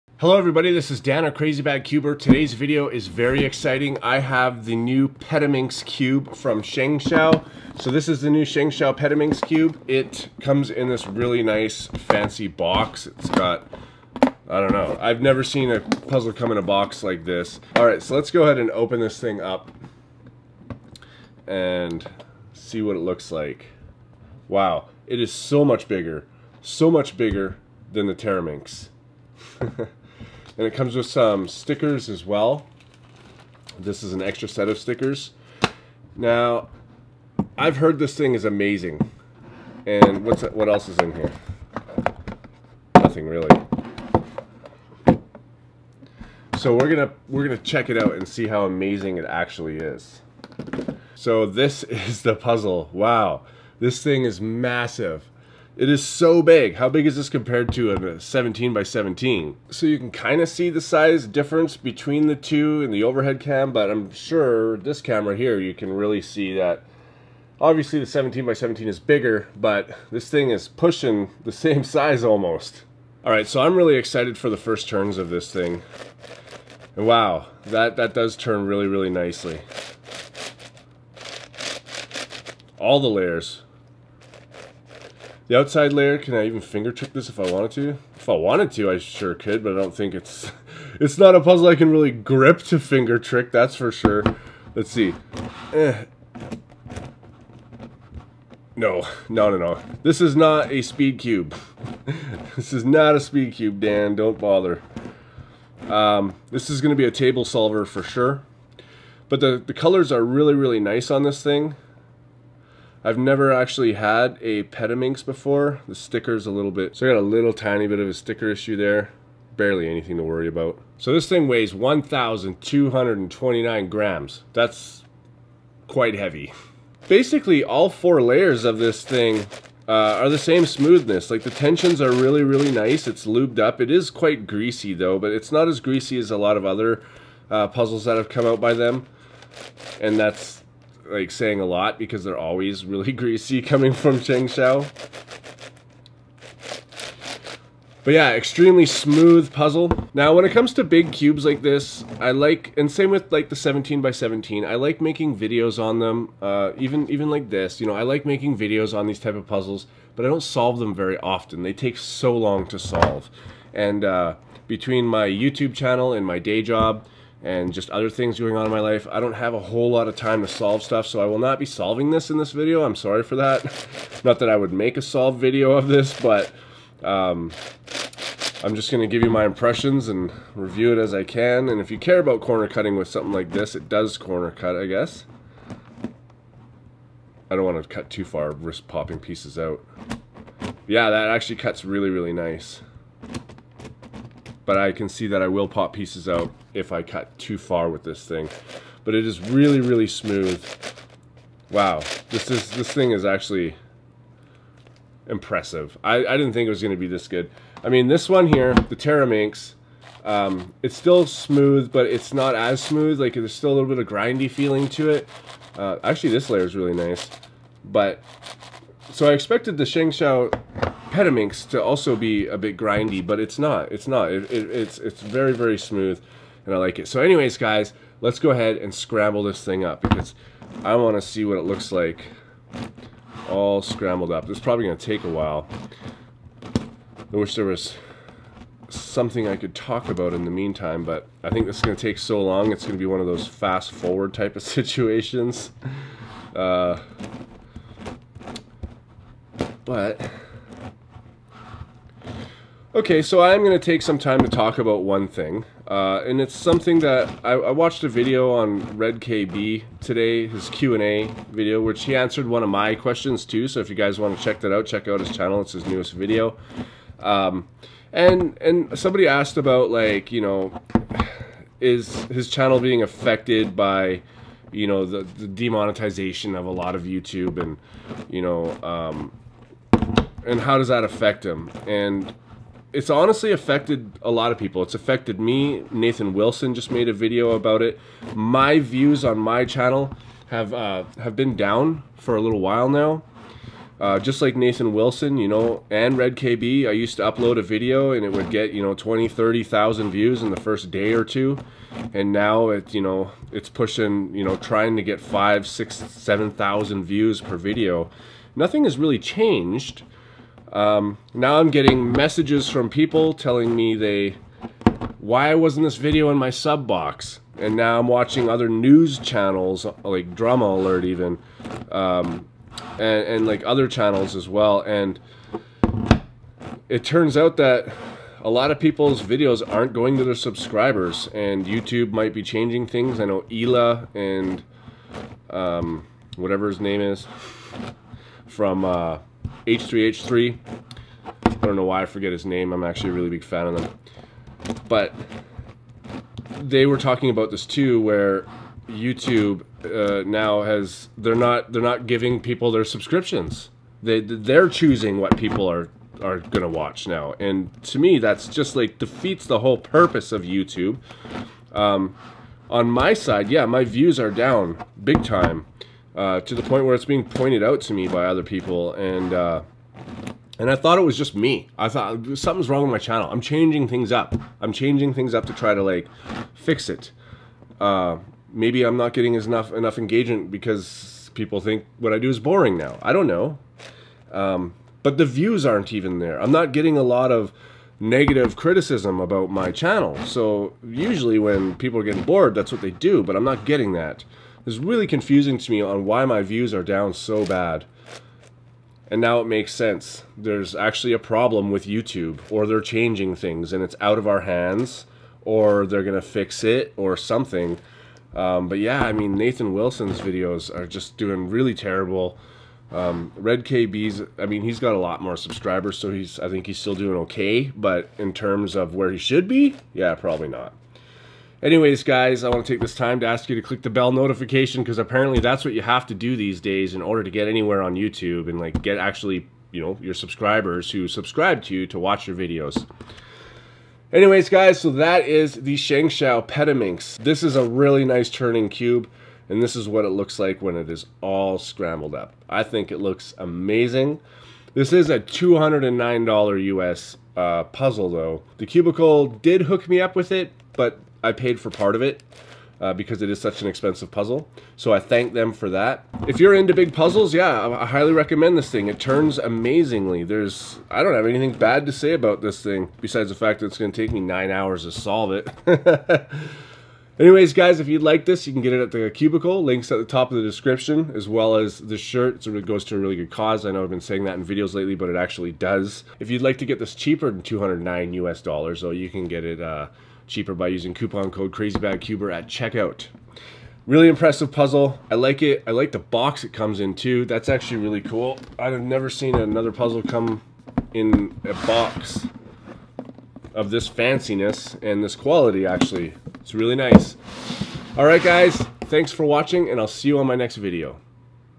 ShengShou Petaminx Unboxing | thecubicle.us
I was super happy that ShengShou made a petaminx that is not pillowed. I much prefer the flat sided puzzles over pillowed puzzles.

This puzzle is not only very smooth, but also extremely stable. I've gone over all the stickers and only the one I showed in the video has a blemish on it, which isn't even a big deal because it comes with extra stickers!!!

If you like big puzzles and want a petaminx, I highly recommend this puzzle!